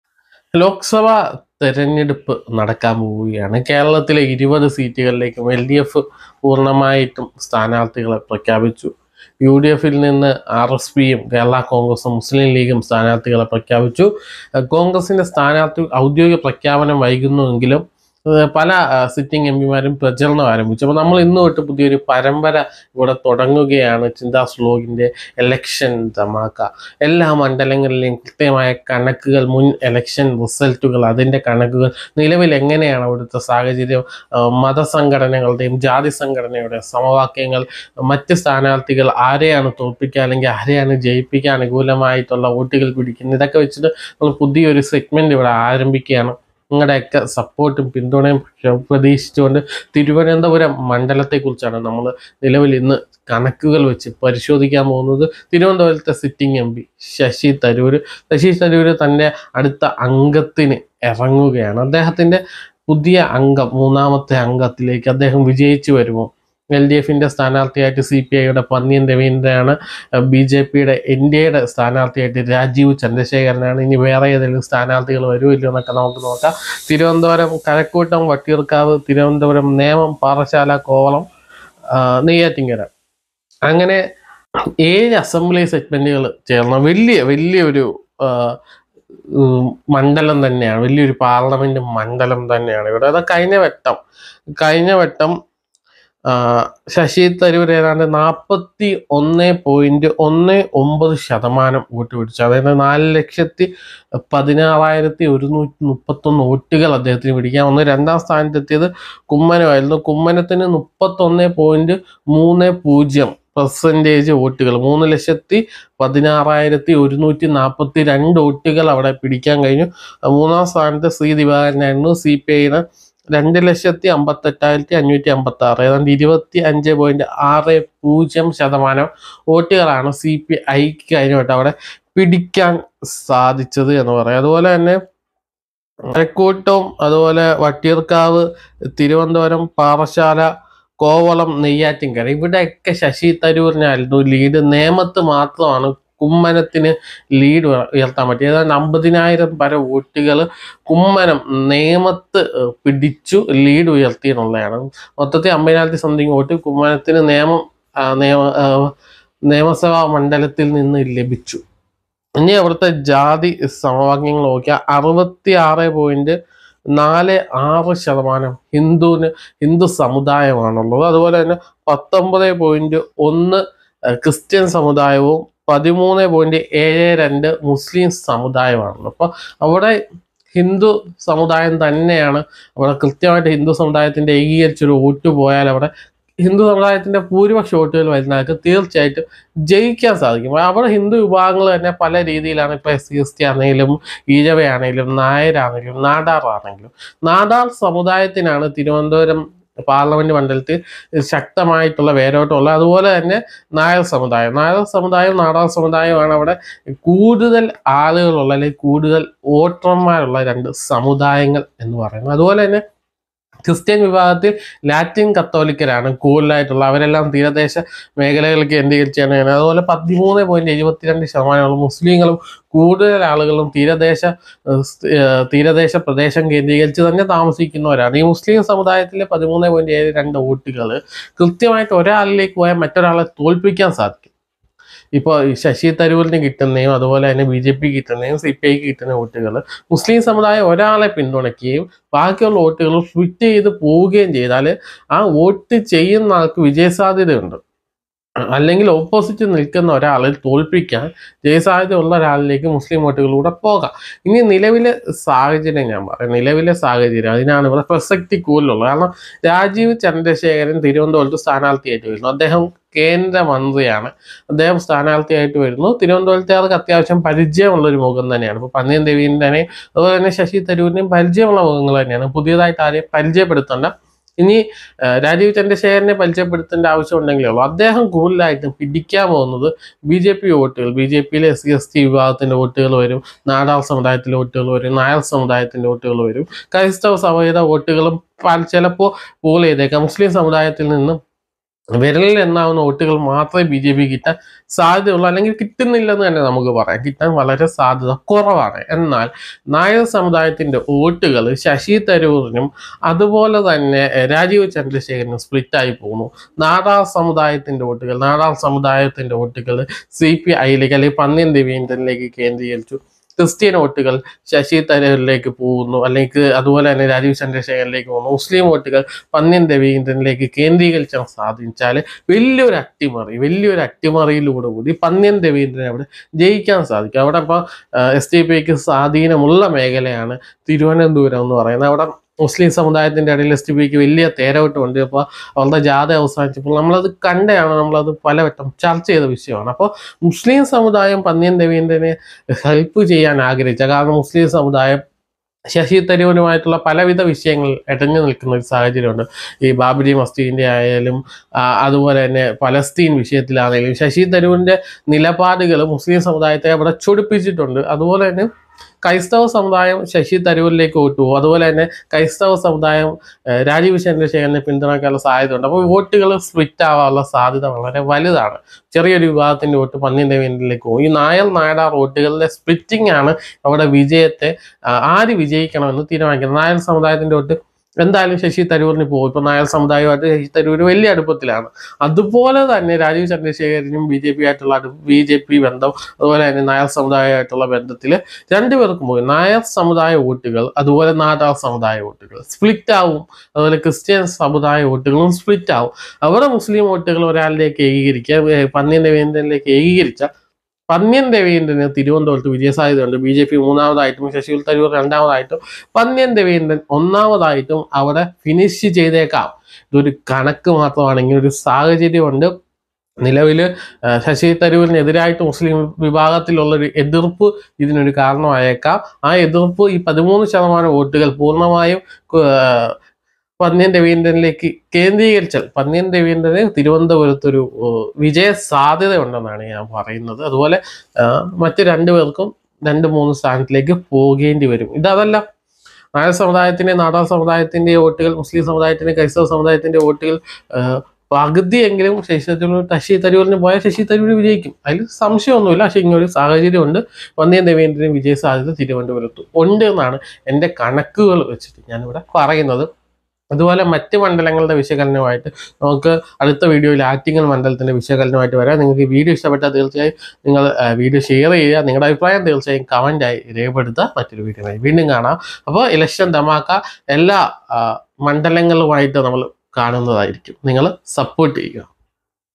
തിരുവനന്തപുരത്ത് മുസ്ലീം വോട്ടുകൾ നിർണ്ണായകം പന്ന്യൻ രവീന്ദ്രന് ജയസാധ്യത - election dhamaka ep:1
chinthaz vlog episode 1 election dhamaka